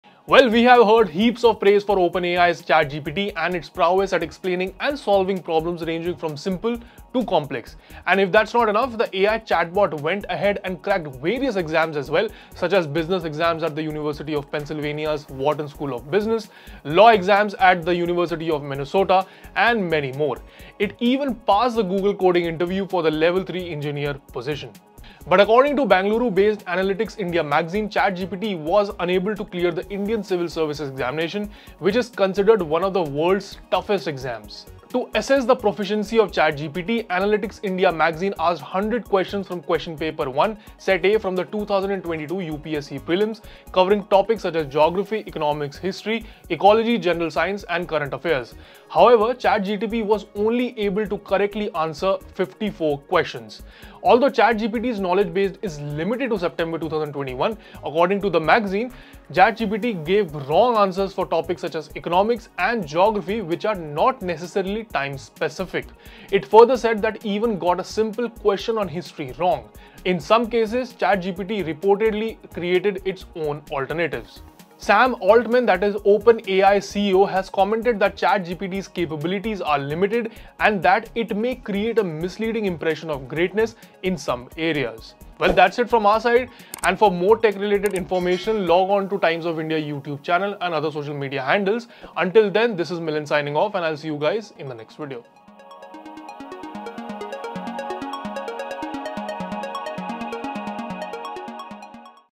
AI chatbot ChatGPT fails to clear UPSC prelims
Open AI's ChatGPT has been praised for its ability to explain and solve problems ranging from simple to complex. However, it was unable to clear the Indian Civil Services Examination, which is considered one of the toughest exams in the world. Analytics India magazine asked 100 questions to ChatGPT, of which it could only answer 54.